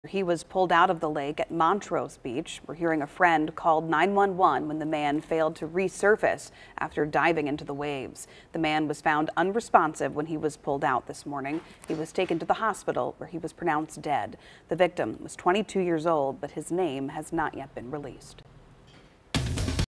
Search for missing swimmer at Evanston beach to resume Monday
A 22-year-old man has died after he was pulled from the water at Montrose Beach Monday morning.